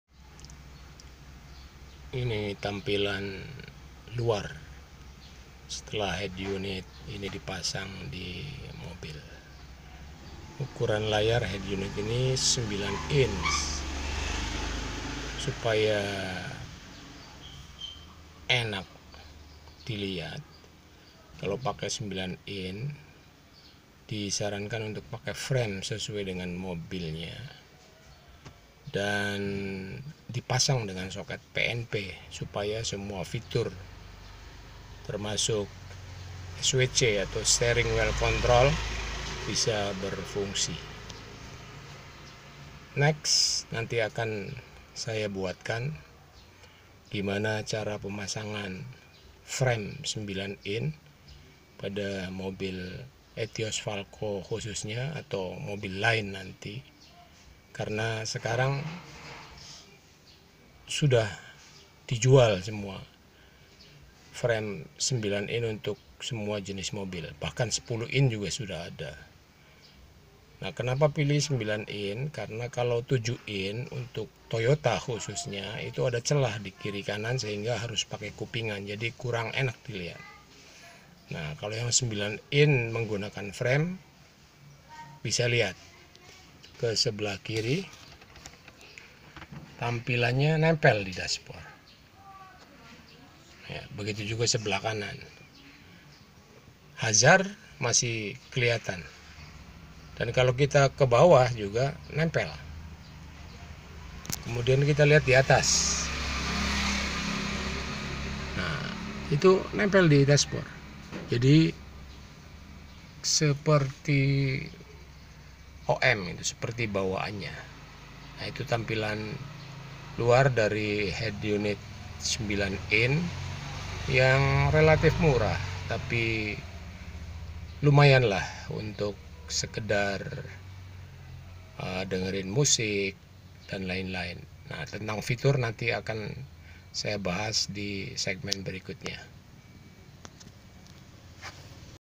Penampakan Head Unit 9 Inch di Etios Valco
Pemasangan Head Unit Android 9 Inch pada etios valco dan mobil lannya agar presisi dan enak dilihat harus menggunakan frame sesuai mobilnya.